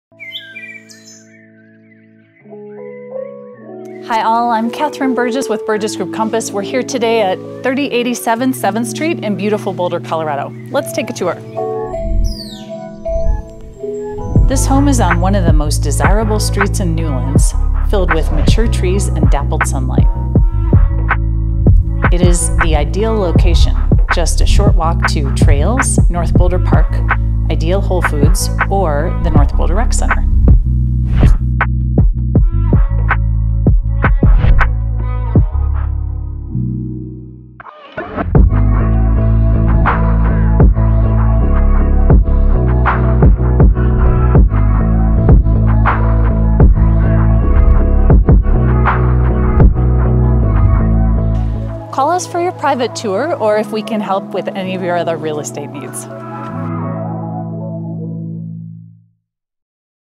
Burgess Group Compass Presents: 3087 7th Street, Boulder
Welcome to 3087 7th St in Boulder, CO.  Airy and bright, this inviting home welcomes you in through gorgeous perennial gardens and abundant patios.  Located on one of the most desirable blocks of Newlands, filled with mature trees, dappled-sunlight and low traffic, this house is a few blocks walk to award-winning schools, the Goat Trail, Ideal Whole Foods, North Boulder Rec Center, and 1.2 miles to Pearl Street. With vaulted ceilings and loads of windows, light streams in even on the cloudiest days. Share mouthwatering meals with friends, while circulating seamlessly between the kitchen, dining room and patios; put your feet up in front of the stylish fireplace. At the end of your day, decompress in your deep tub with a glass of wine, then wake up peek-a-boo views of the Flatirons. Well-maintained and updated throughout the years, this home is in move-in condition. Showings start Friday, June 4th.